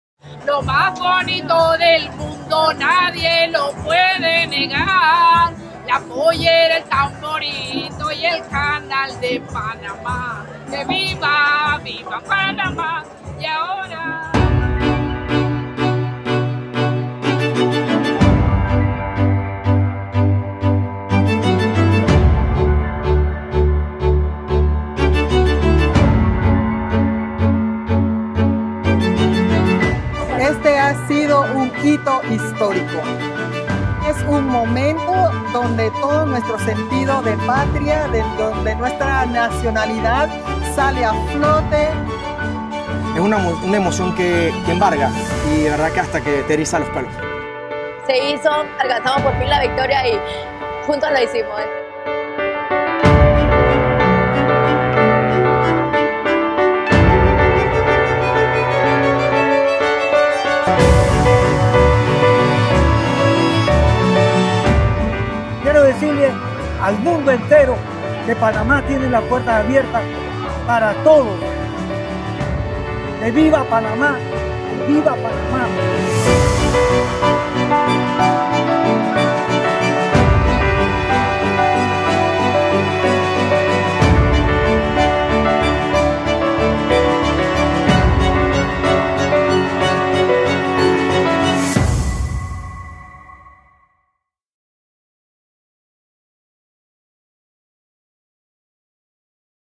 La emoción de vivir la Inauguración del Canal Ampliado
Más de 20 mil panameños y panameñas vivieron parte de la historia el 26 de junio en la inauguración del Canal Ampliado.